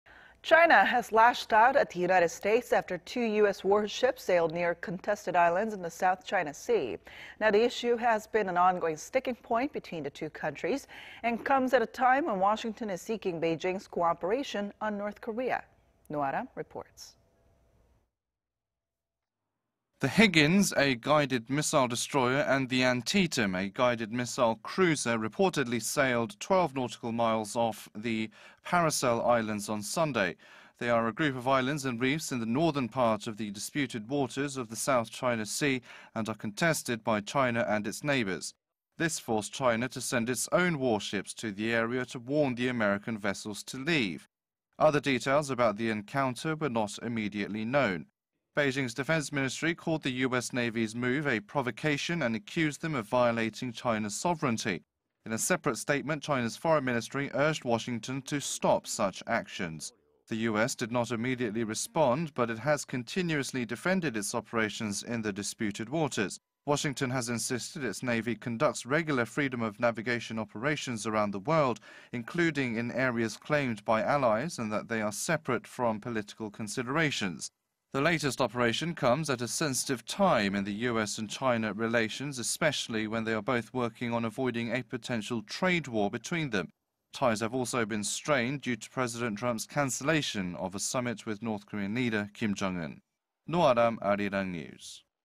China denounces U.S. warships' entry into South China Sea as 'provocation'
美함정 2척, 남중국해 '항행의 자유' 작전…中과 군사갈등 고조
  China has lashed out at the United States after two U.S. warships sailed near contested islands in the South China Sea.
The issue has been an on-going sticking point between the two nations... and comes at a time when Washington is seeking Beijing's cooperation on North Korea.
Ro Aram reports.
  The Higgins - a guided-missile destroyer - and the Antietam - a guided-missile cruiser reportedly sailed twelve nautical miles of the Paracel Islands on Sunday.
They are a group of islands and reefs in the northern part of the disputed waters of the South China Sea and are contested by China and its neighbors.
This forced China to send its own warships to the area to warn the American vessels to leave.
Other details about the encounter were not immediately known.
Beijing's defense ministry called the U.S. Navy's move a provocation and accused them of violating China's sovereignty.
In a separate statement, China's foreign ministry urged Washington to stop such actions.
  The U.S. did not immediately respond, but it has continuously defended its operations in the disputed waters.
Washington has insisted its Navy conducts regular freedom of navigation operations around the world, including in areas claimed by allies, and that they are separate from political considerations.
The latest operation comes at a sensitive time in U.S. and China relations, especially when they are both working on avoiding a potential trade war between them.
  Ties have also been strained due to President Trump's cancellation of a summit with North Korean leader Kim Jong-un.
Ro Aram, Arirang News.